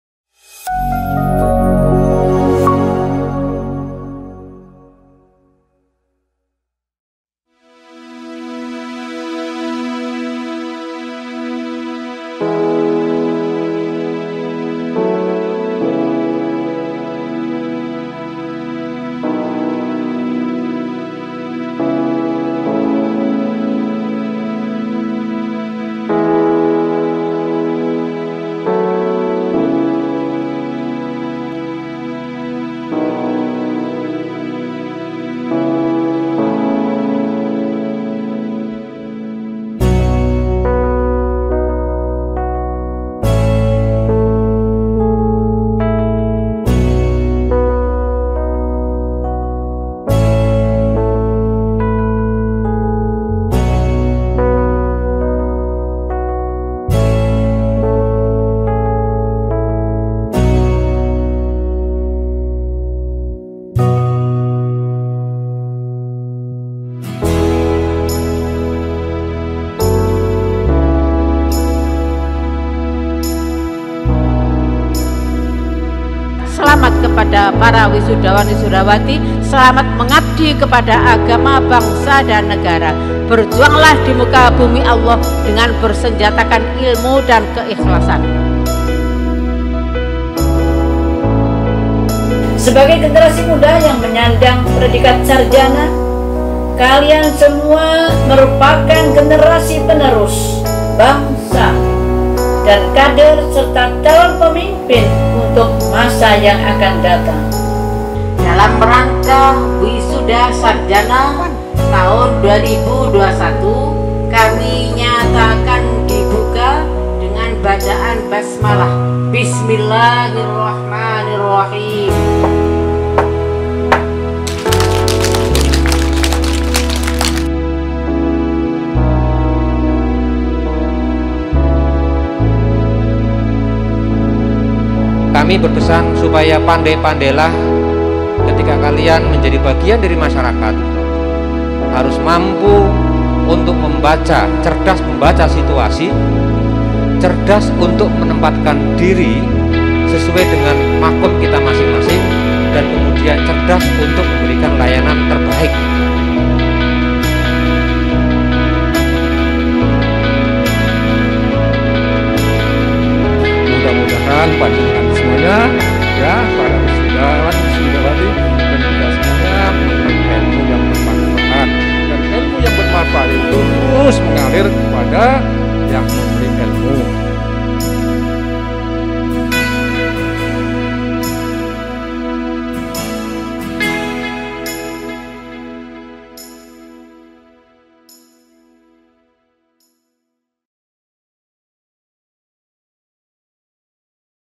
Trailer Wisuda Sarjana STIT-UW Jombang Tahun 2021
STIT Al-Urwatul Wutsqo Jombang Terakreditasi dengan predikat "BAIK"
1. Pendidikan Agama Islam (PAI) Terakreditasi B
2. Manajemen Pendidikan Islam (MPI) Terakreditasi C
Facebook: Stit Al Urwatul Wutsqo
Wisuda ini dilaksanakan pada hari ahad tanggal 13 Juni 2021